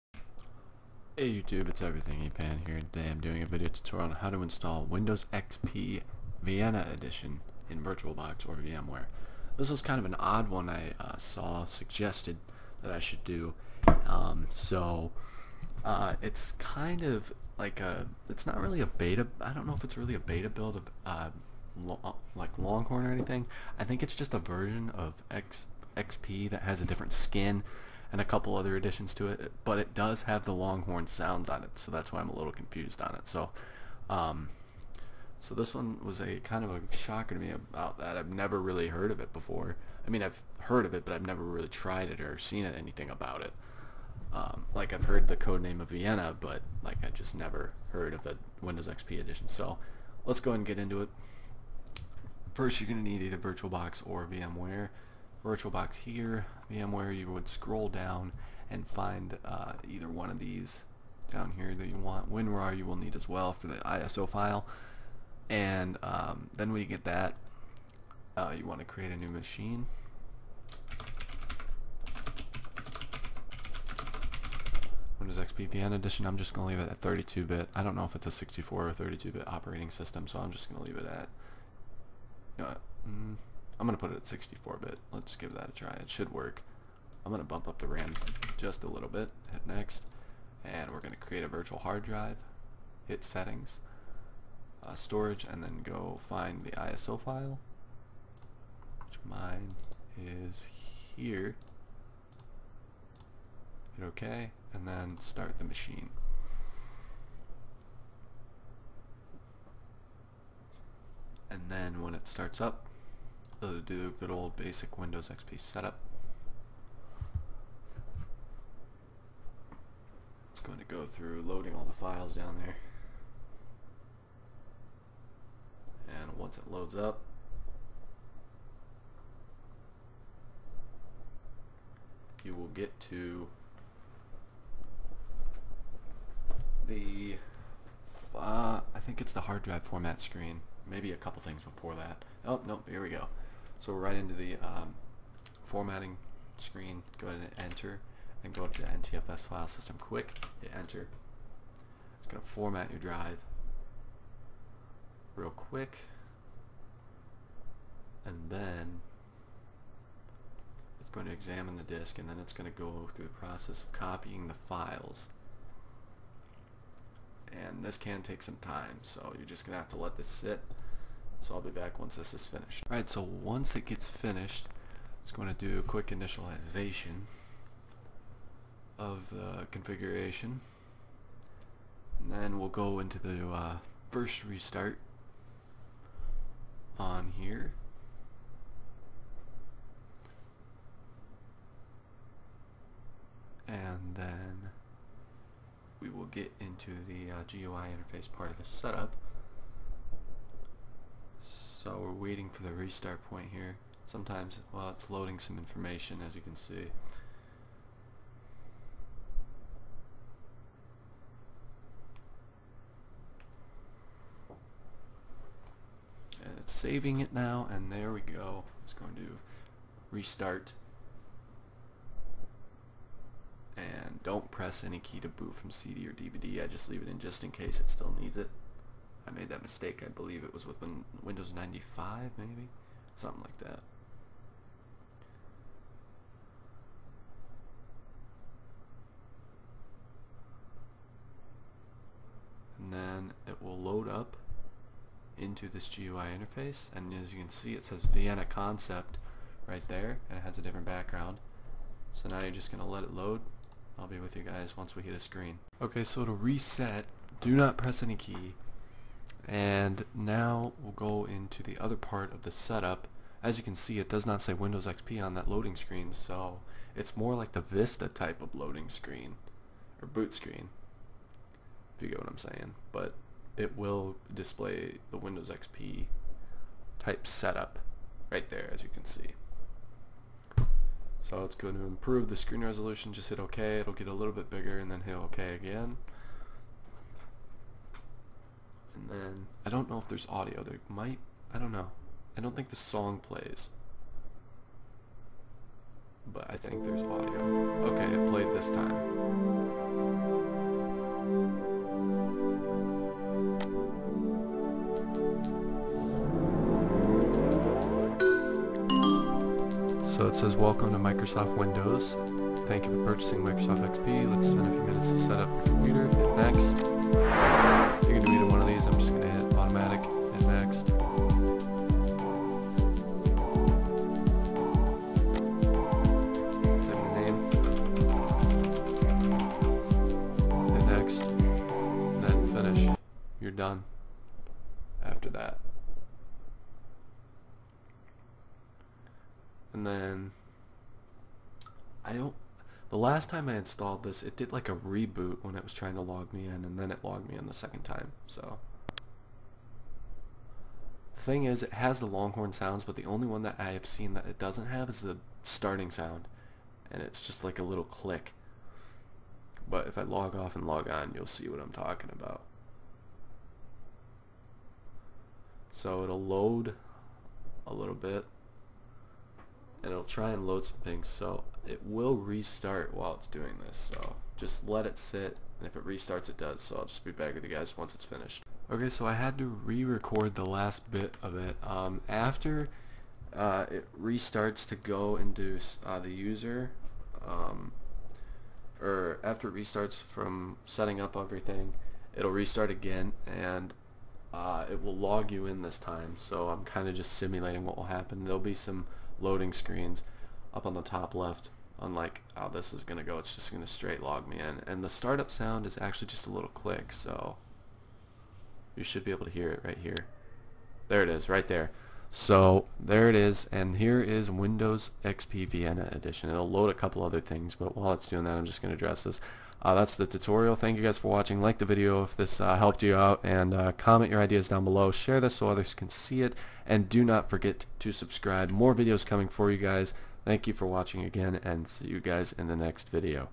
Windows XP Vienna Edition - Installation in Virtualbox
Here is a video tutorial on how to install Windows XP Vienna Edition in Virtualbox!  This one I never really knew about so maybe it will be new to some of you!